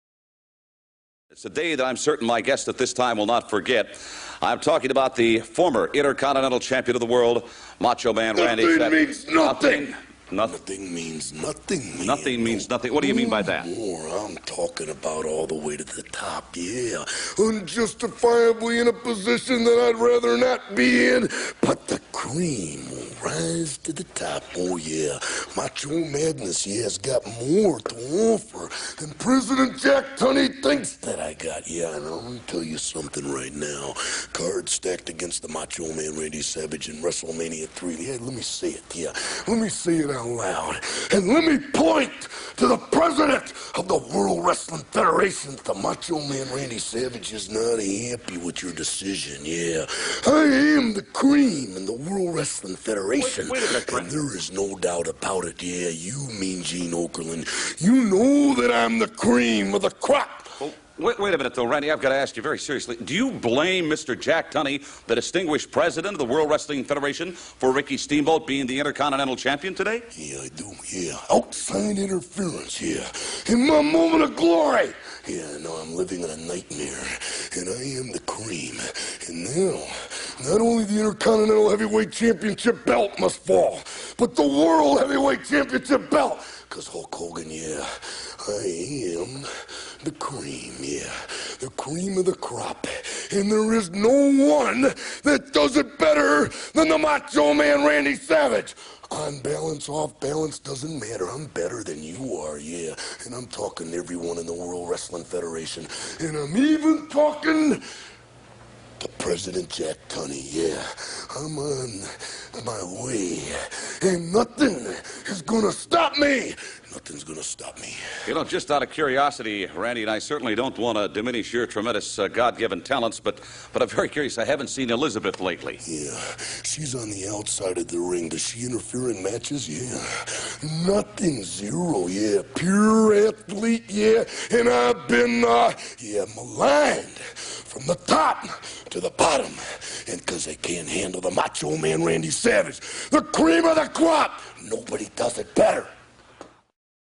Macho Man & Mean Gene - The Cream Rises to the Top
Macho Man & Mean Gene discuss Macho Man losing the title, but Macho Man reassures everyone that the cream (non dairy creamer in hand) rises to the top!